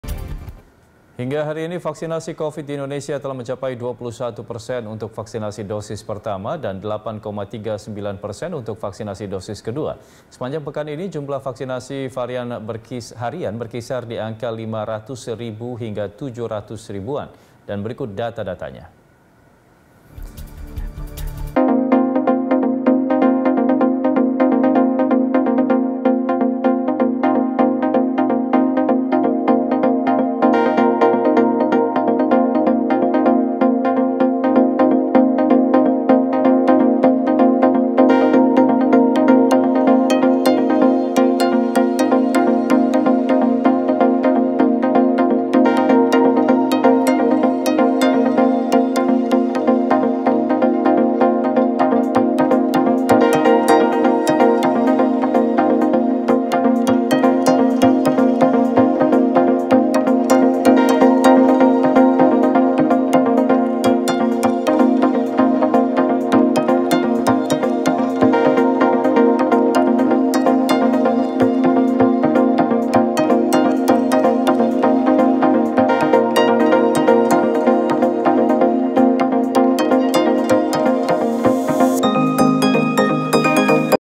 Target Vaksinasi di Indonesia Hingga 24 Juli 2021
Hingga hari ini, vaksinasi COVID di Indonesia telah mencapai 21 persen untuk vaksinasi dosis pertama dan 8,39 persen untuk vaksinasi dosis kedua. Sepanjang pekan ini, jumlah vaksinasi harian berkisar di angka 500 ribu hingga 700 ribuan.